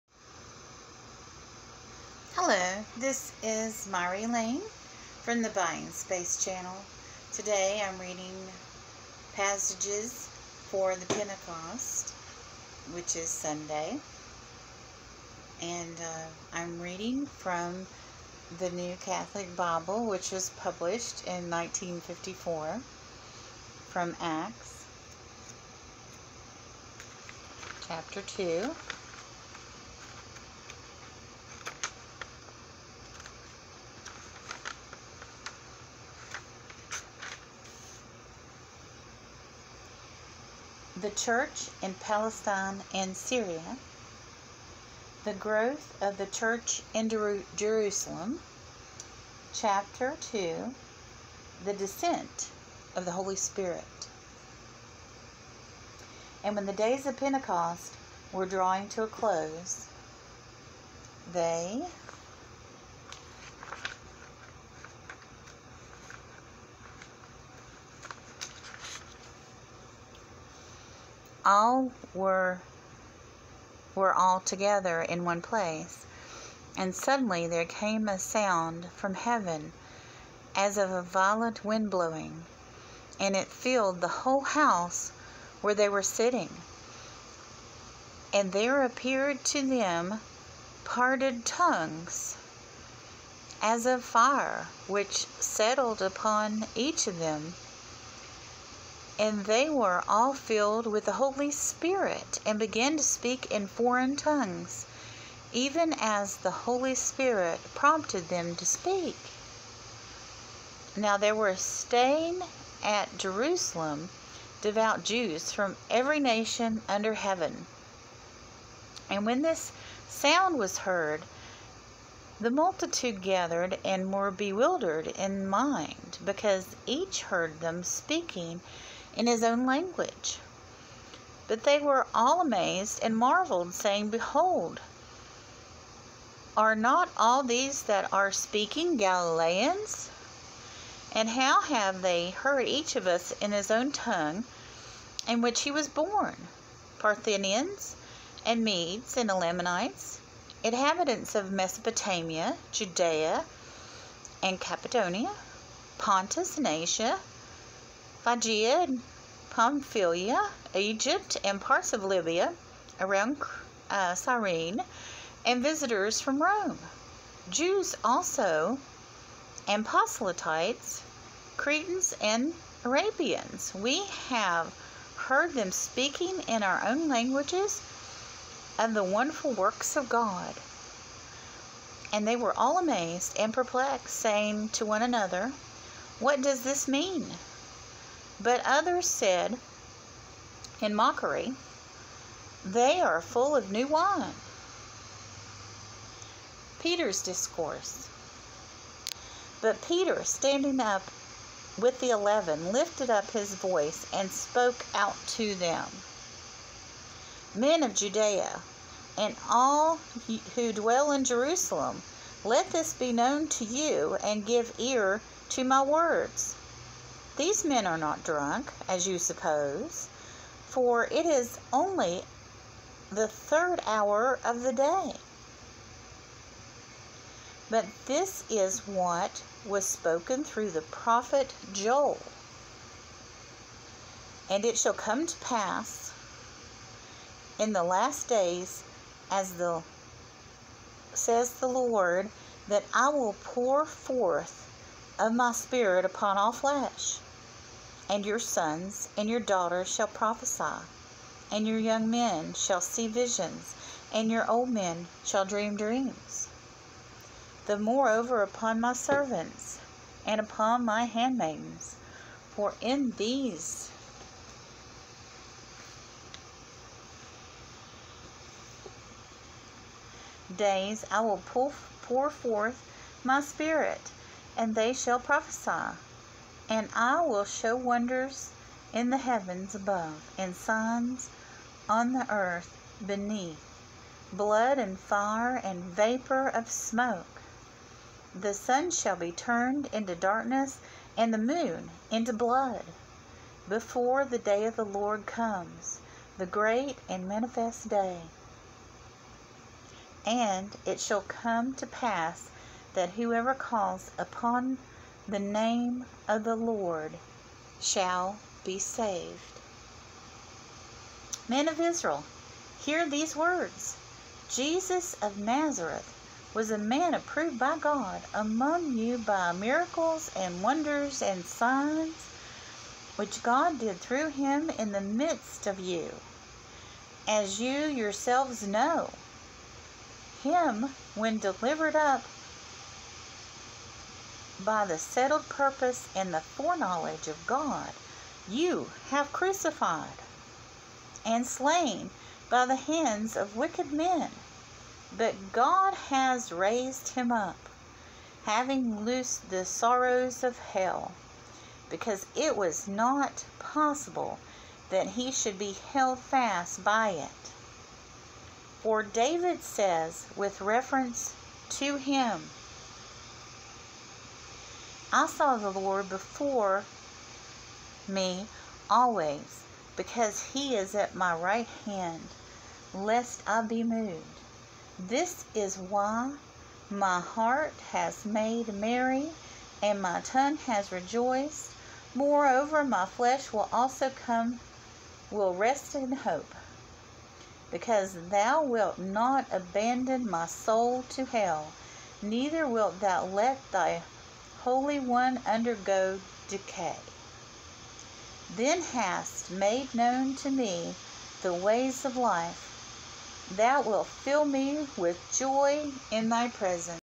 Acts 2 : 1-28 The Pentacost
The Holy Spirit fills Christian hearts for the first time. Peter speaks the prophecy of Joel and David.